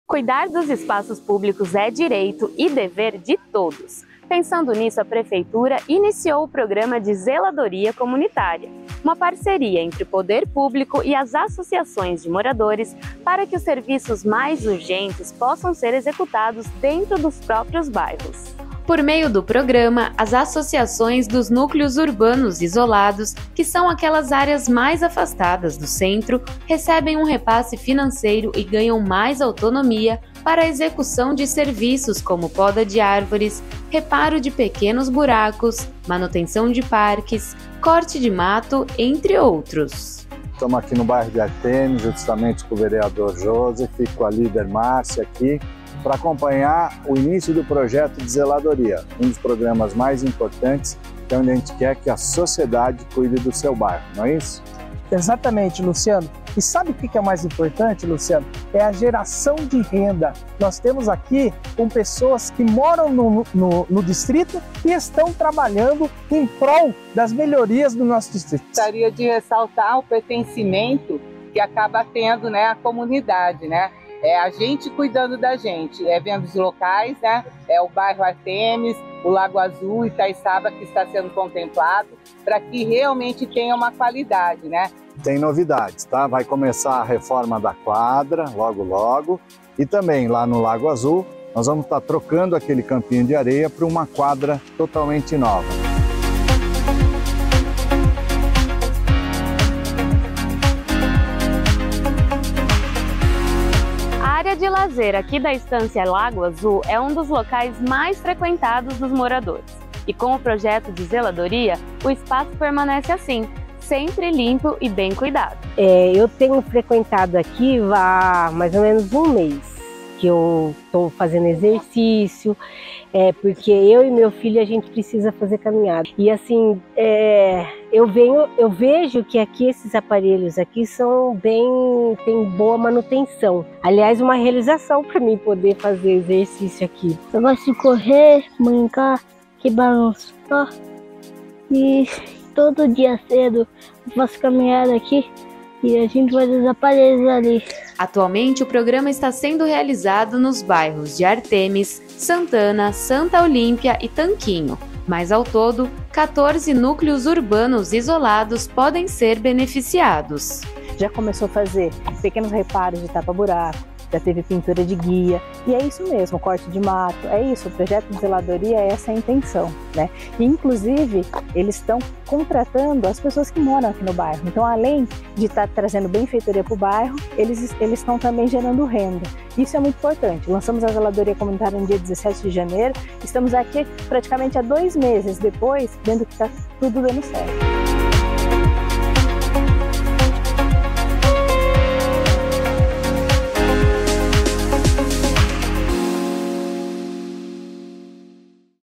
Zeladoria comunitária: prefeito e secretária de Governo acompanharam serviços em Artemis
A Prefeitura esteve ontem no bairro de Artemis, acompanhando as primeiras ações do Programa de Zeladoria Comunitária. ✅

Dê o play para conferir como foi!